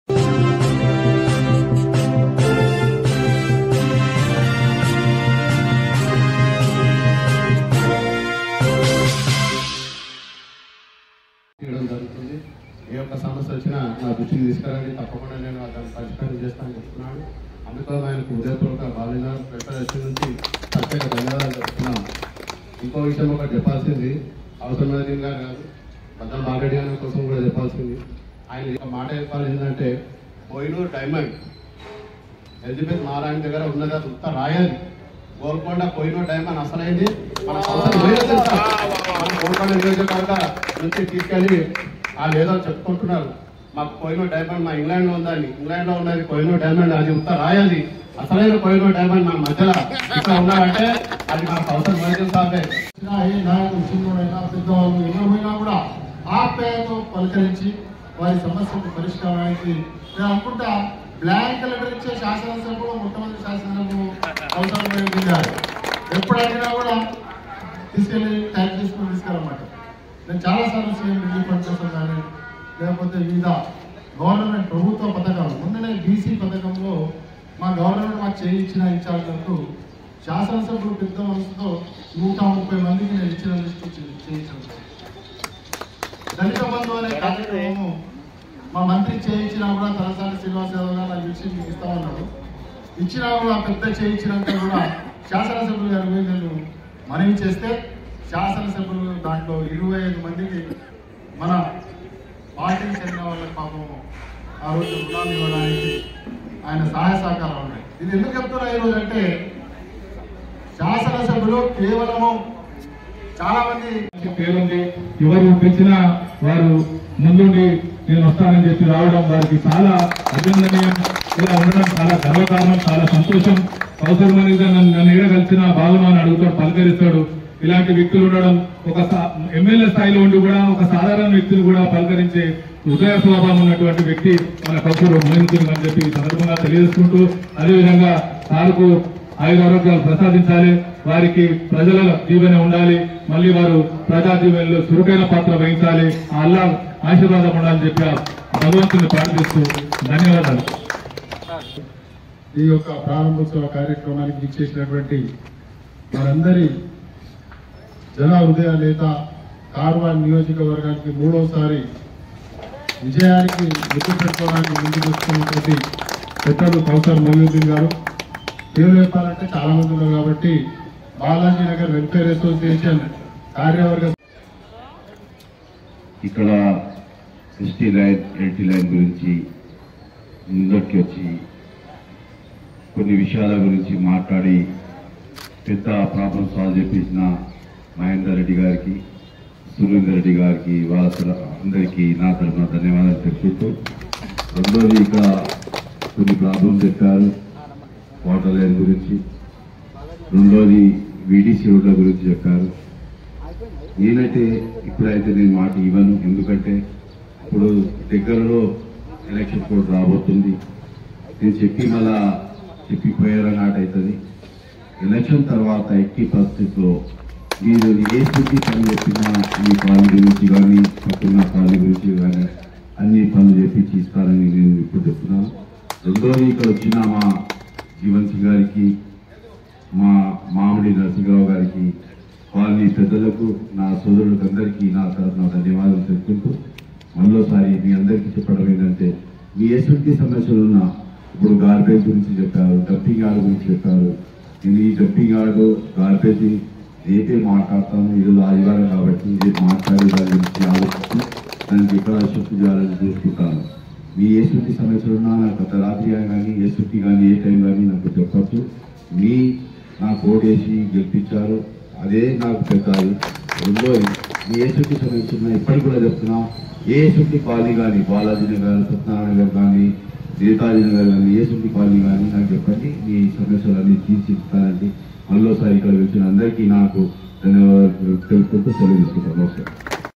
Today AIMIM Karwan MLA Kausar Mohiuddin Sahab Inaugurated 33kv HT Electrical Lines At Balaji Nagar
33kv HT Electrical Lines Inaugurated In Karwan Constituency !!!

Under The Leadership Of AIMIM President Barrister Asaduddin Owaisi Sahab

Today AIMIM Karwan MLA Kausar Mohiuddin Sahab Inaugurated 33kv HT Electrical Lines At Balaji Nagar Guddimalakapur In Karwan Constituency.

MLA Sahab Accompanied By AIMIM Primary Unit President &  Active Workers And Areas Residents Were Present.